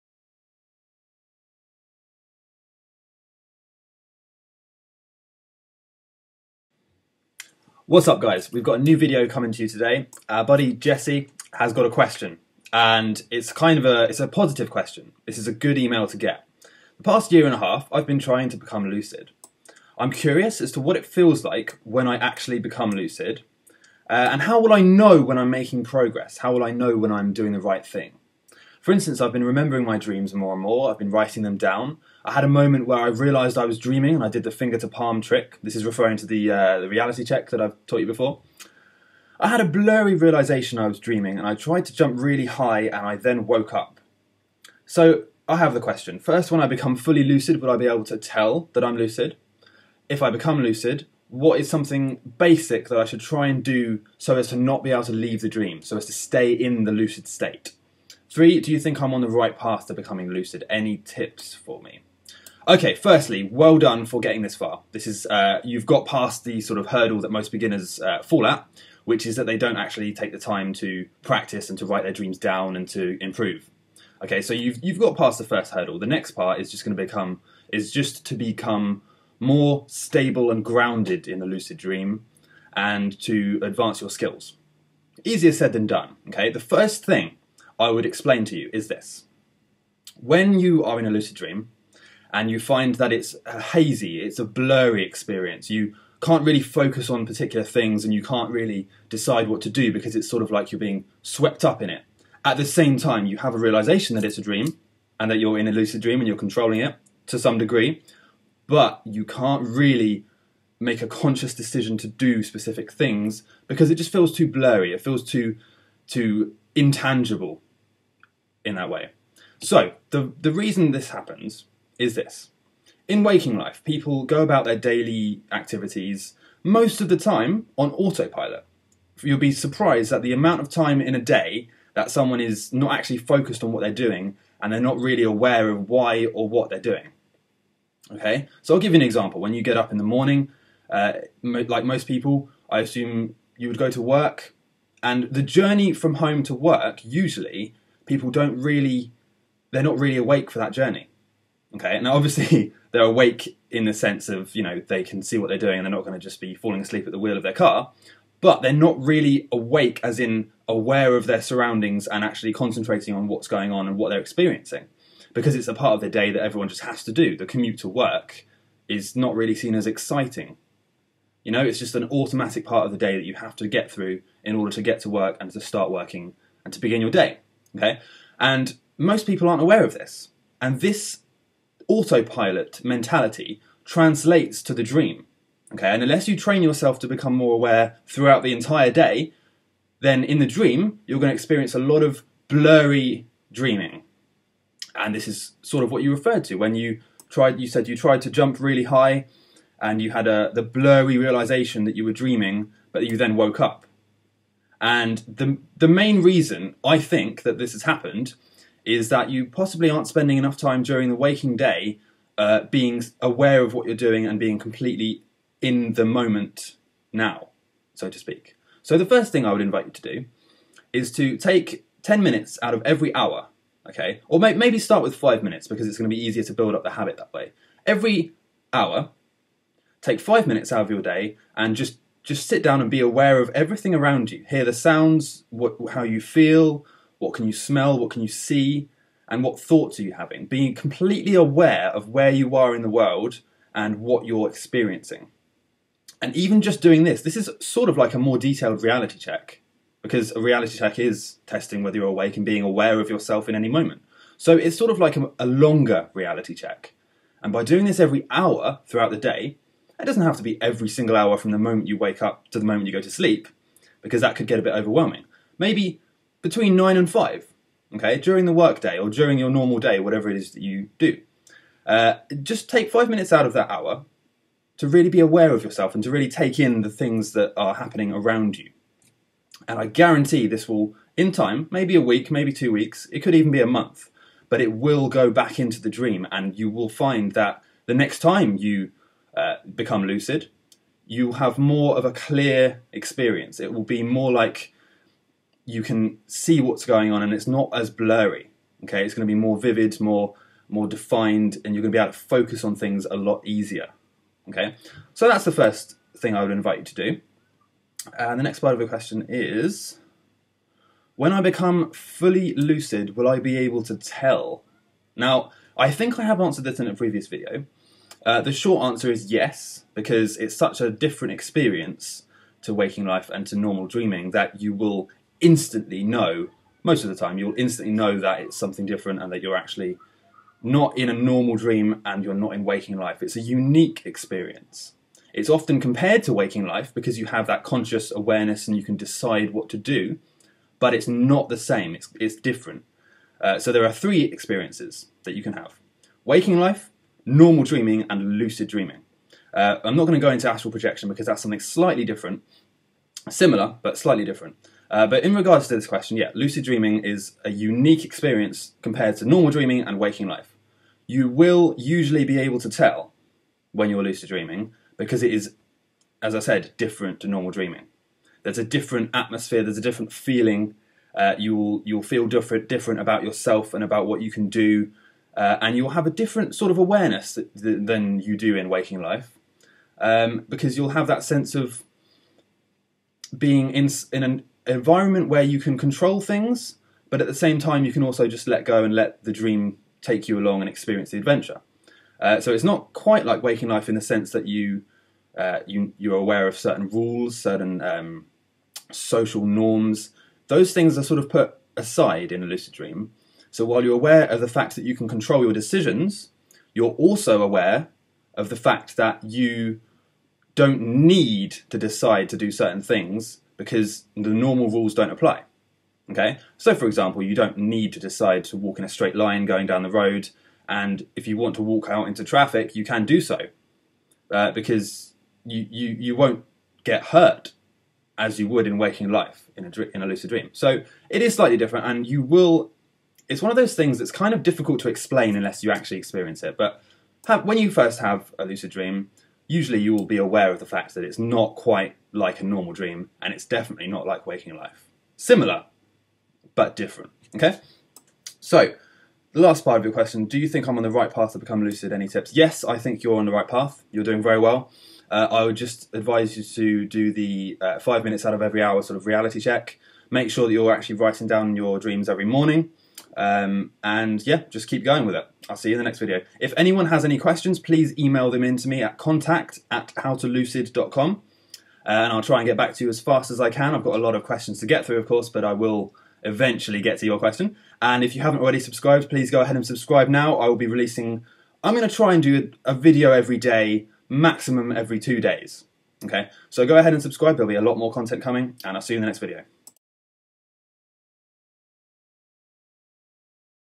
Growing As A Dreamer (What To Do NEXT) - HowToLucid.com
Jesse asks a few questions. Firstly, how do I remove the 'blurry' sensation in a lucid dream, as it seems when I become Lucid I can't see clearly and it's all blurry.

Secondly, When I become lucid, how will I be able to KNOW that I'm lucid? What does it feel like?

Thirdly, am I on the right track? How to know when you're making progress, and what should I do NEXT?